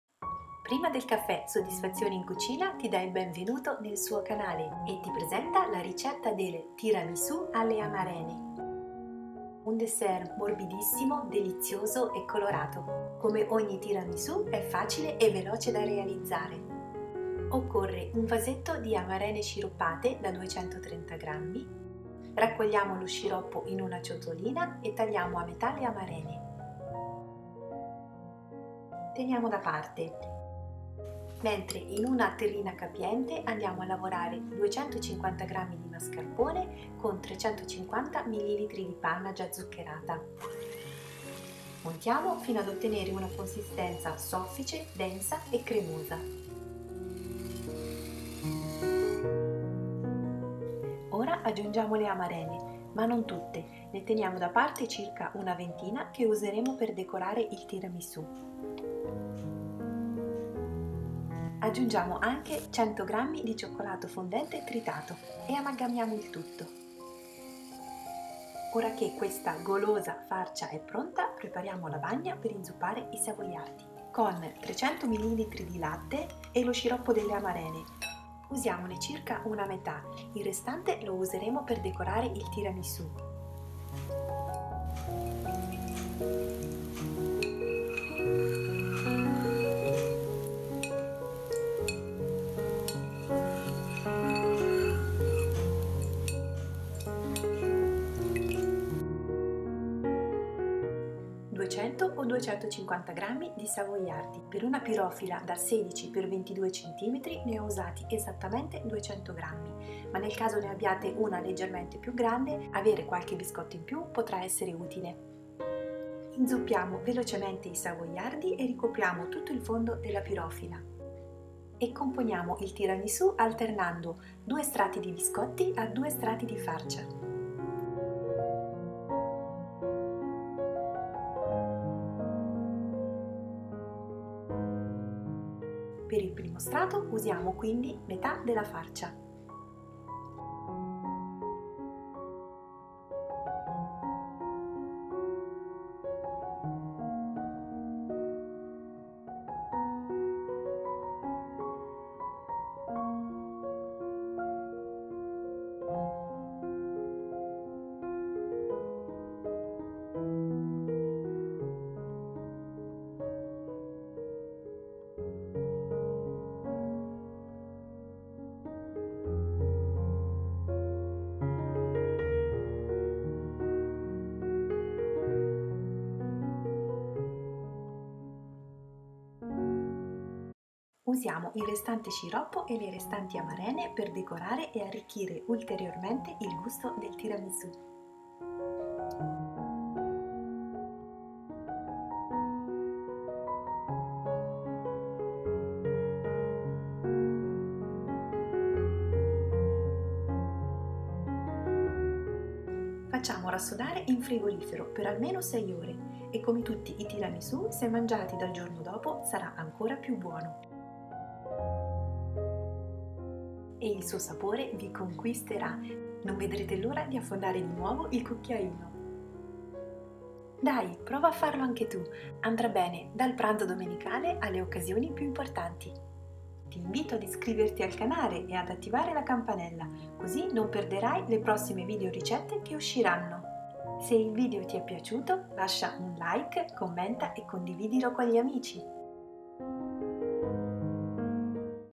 TIRAMISU ALLE AMARENE facile e buonissimo - tiramisu with sour cherries
Il tiramisù alle amarene, facile, delizioso e colorato, saprà conquistare il vostro palato. Potrai proporlo dal pasto domenicale alle occasioni più importanti.  🇬🇧 ingredients ⬇️

🇮🇹 INGREDIENTI per 8 porzioni
* 1 vasetto di amarene sciroppate da 230 gr
* 250 gr di mascarpone
* 350 ml di panna zuccherata
* 100 gr di cioccolato fondente
* 300 ml di latte
* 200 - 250 gr di savoiardi
* pirofila 16 x 22 cm

* 230 grams of sour cherries syrup
* 250 grams of mascarpone cheese
* 350 ml of sweetened whipping cream
* 100 grams of dark chocolate
* 300 ml of milk
* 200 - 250 grams of savoiardi biscuits
* mold 16 x 22 cm

★ Potete seguire Prima Del Caffè Soddisfazioni in cucina anche su ★

Ultimo video: "usa la FRUTTA FRESCA DI STAGIONE e fai il DOLCE FREDDO ALLA FRUTTA dessert FACILISSIMO e TANTO BUONO"